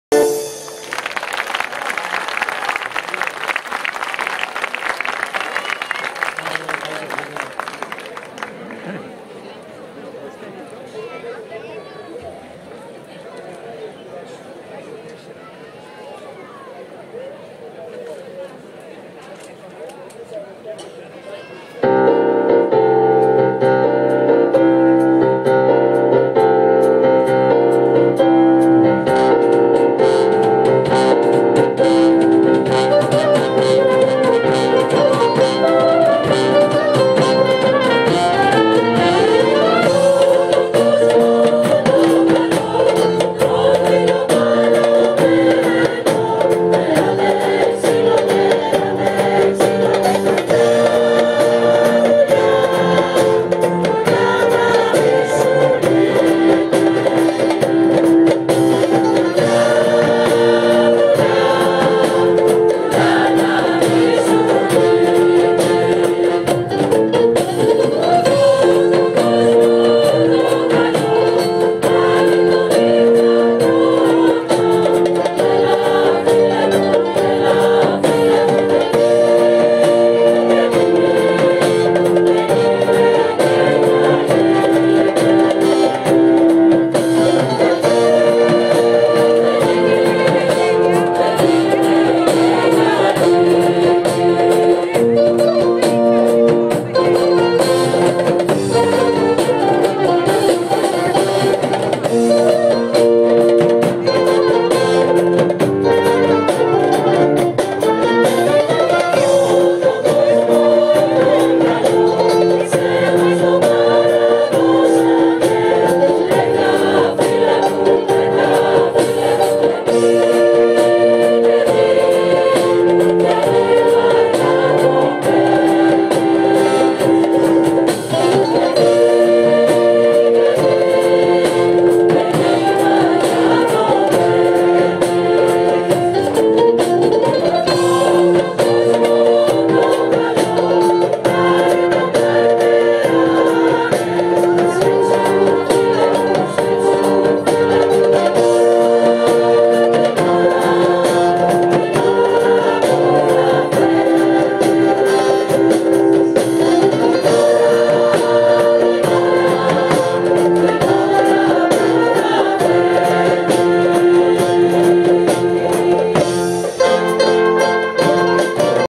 Συναυλία στο πλαίσιο των εγκαινίων της 87ης Εμποροπανήγυρης Αιγινίου 2013
Απόσπασμα από τη συναυλία στο πλαίσιο των εγκαινίων της 87ης Εμποροπανήγυρης Αιγινίου 2013